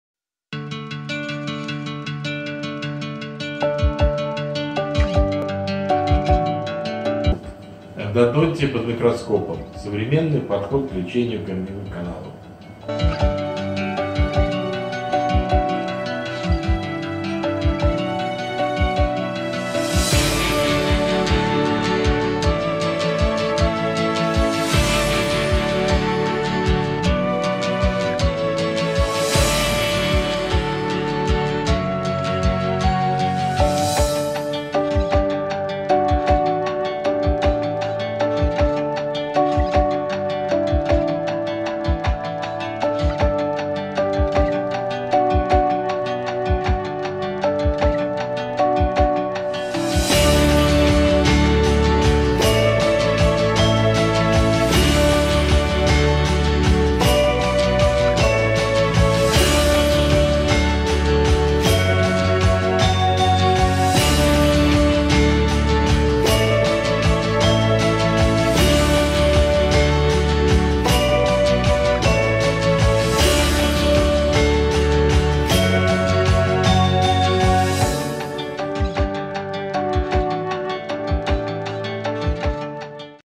Лечение зубов под микроскопом
Кандидат медицинских наук, ведущий врач стоматолог-терапевт клиник "Мегастом" Мощиль Андрей Иванович о современном подходе к лечению корневых каналов.